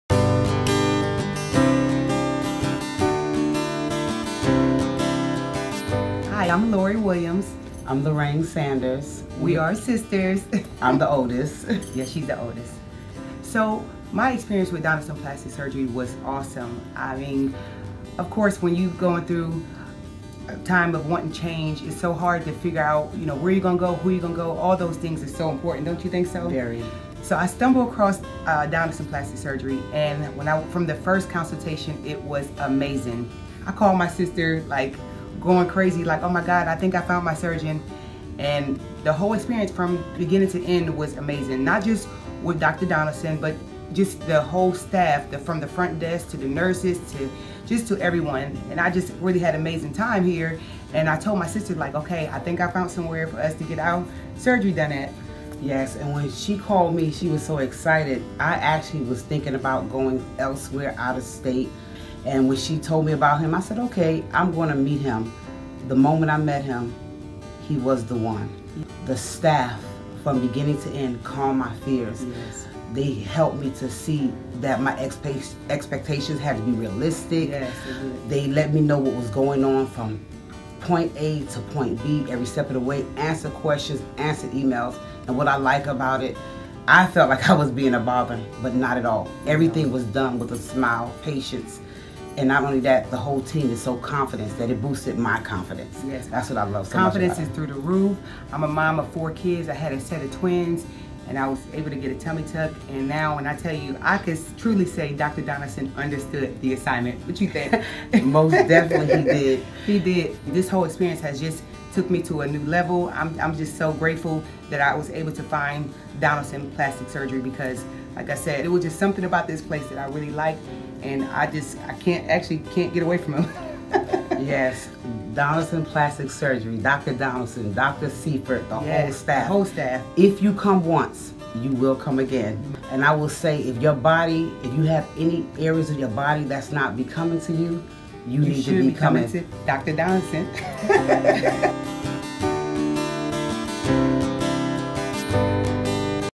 "Dr. Donaldson Understood the Assignment" According to Tummy Tuck Patients
Lori and Lorain are sisters who underwent tummy tucks together! In this testimonial video, they discuss the impact that Donaldson Plastic Surgery and Dr. Donaldson have had on their confidence.